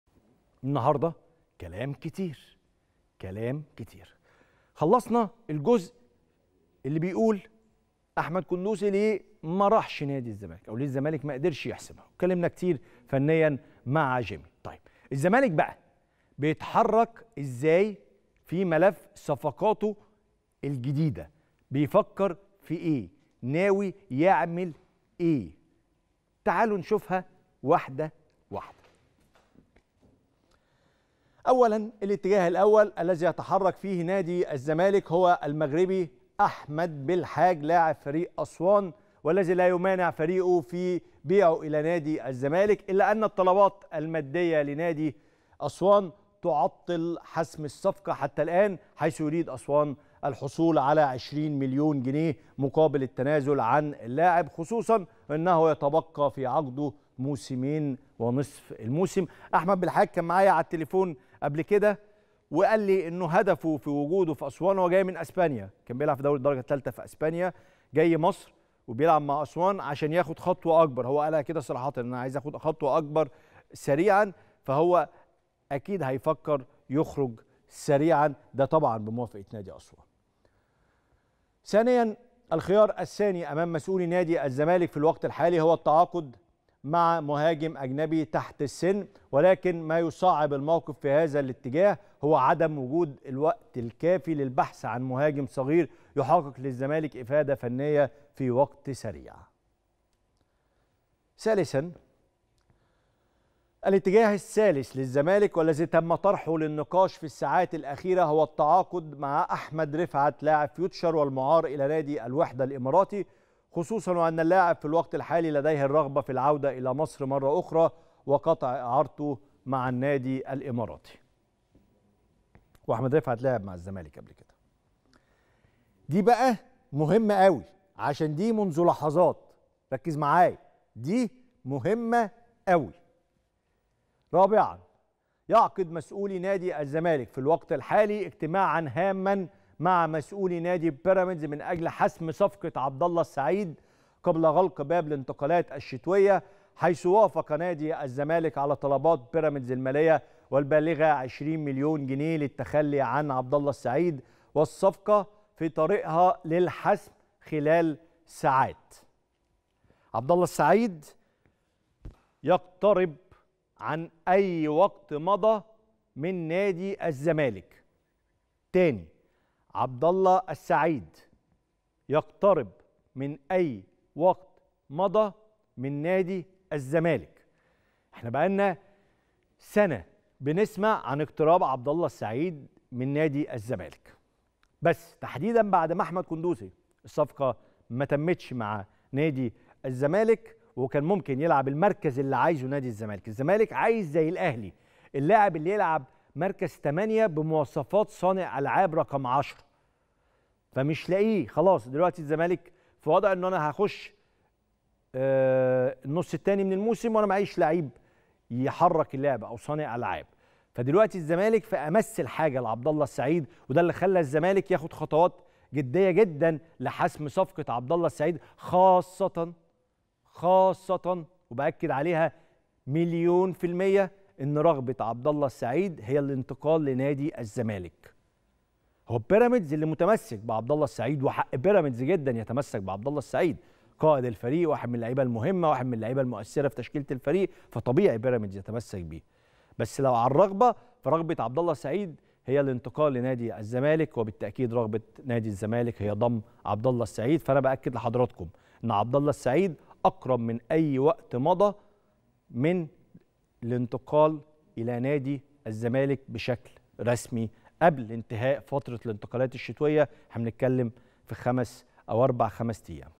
نجم بيراميدز .كيف يتحرك الزمالك في ملف الصفقات الجديدة ؟ كواليس مثيرة يكشفها كريم رمزي
رقم 10 | نجم بيراميدز ولاعب الفريق السابق ..كيف يتحرك الزمالك في ملف الصفقات الجديدة الآن؟ كواليس مثيرة يكشفها كريم رمزي